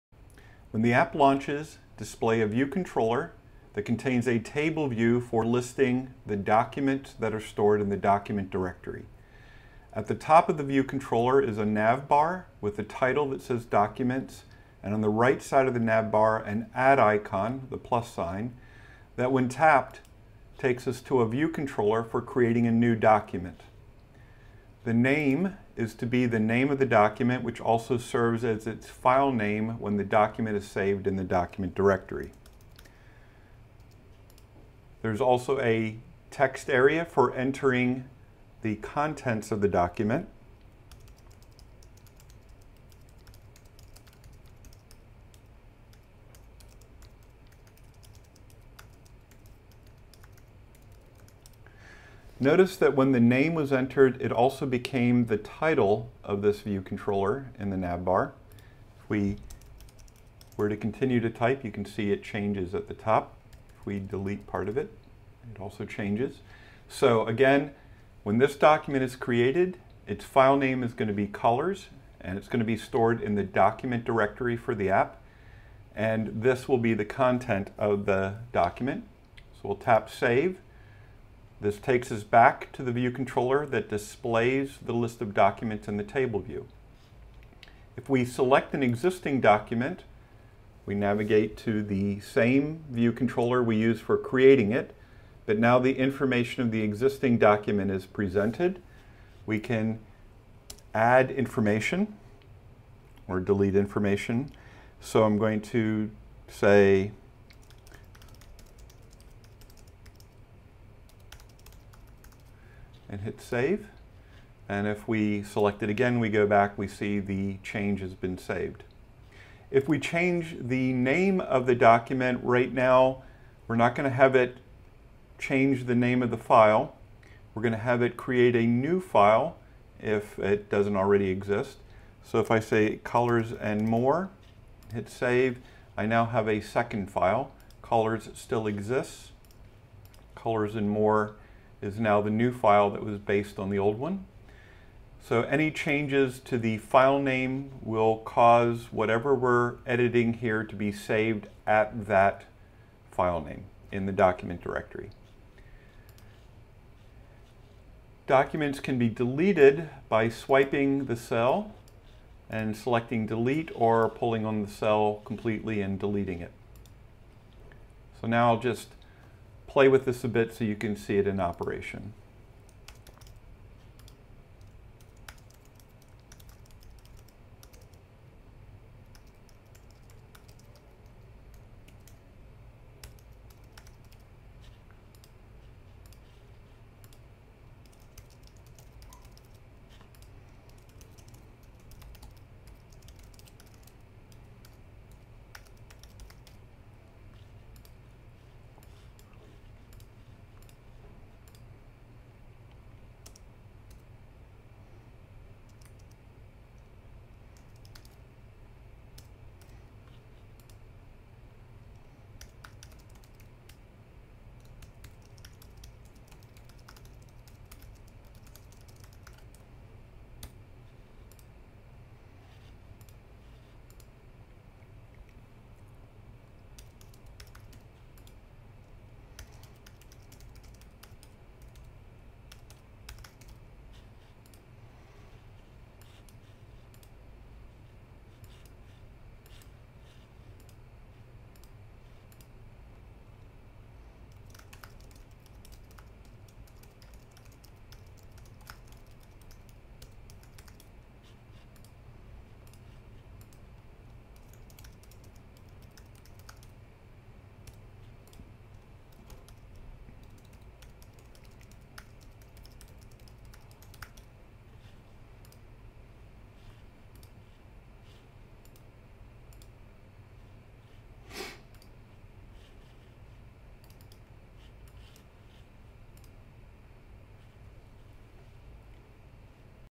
Documents Demo
A demonstration of the Documents app to be created for version 1.